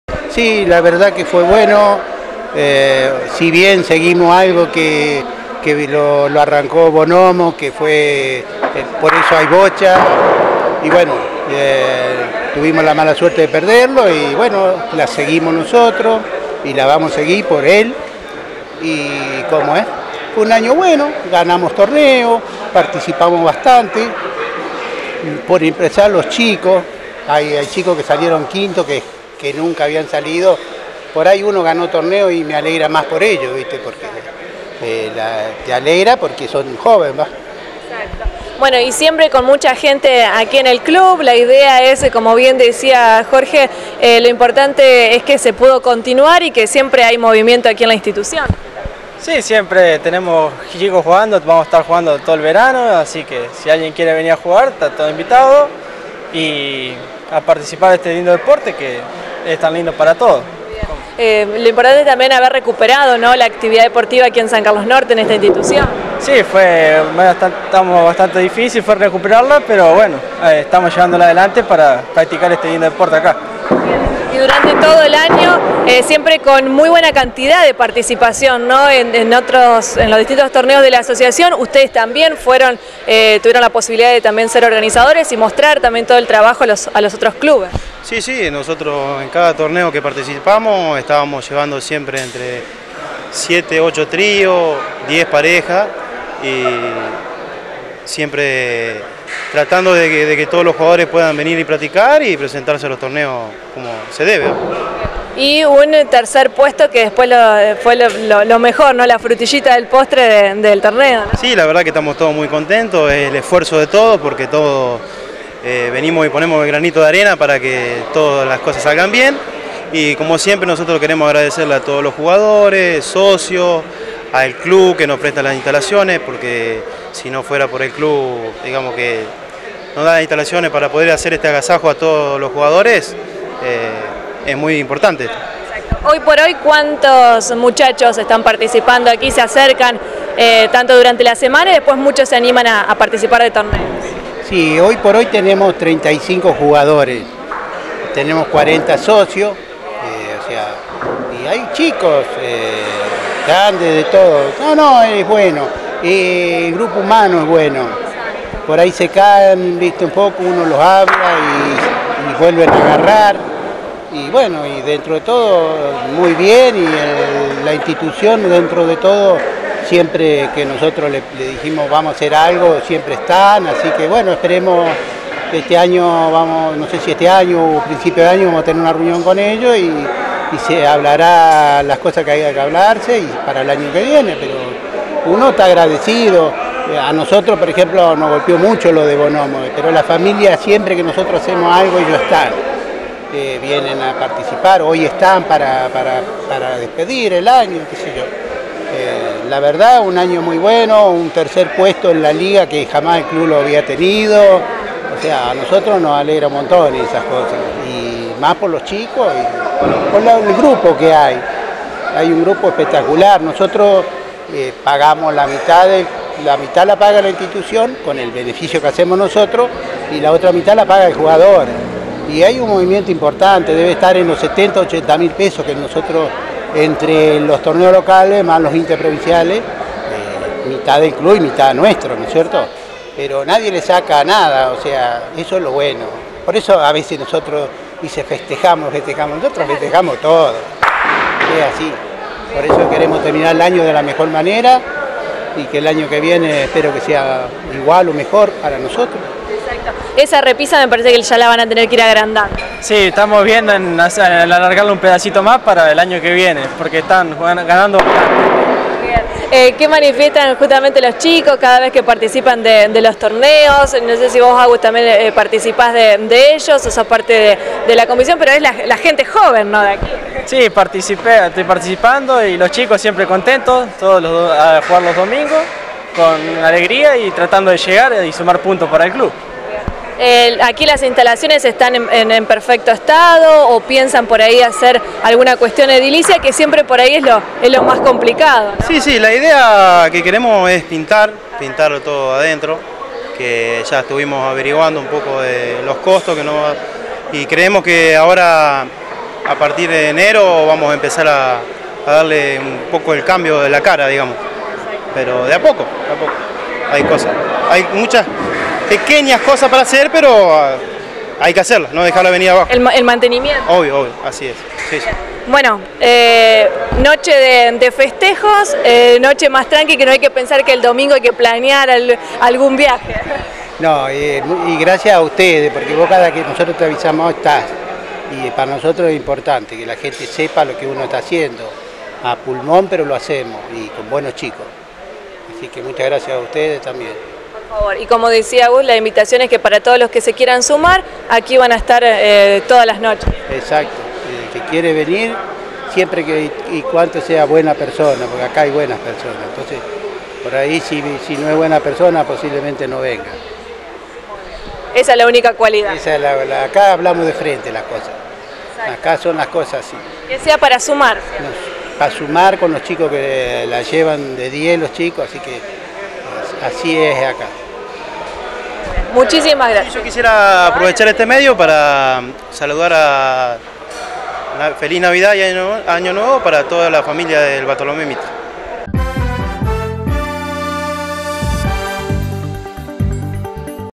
JORGE RAMOS – AGUSTÍN REY – DANIEL SOTO
LOS BOCHOFILOS DEL BARTOLOMÉ MITRE DESPIDIERON EL AÑO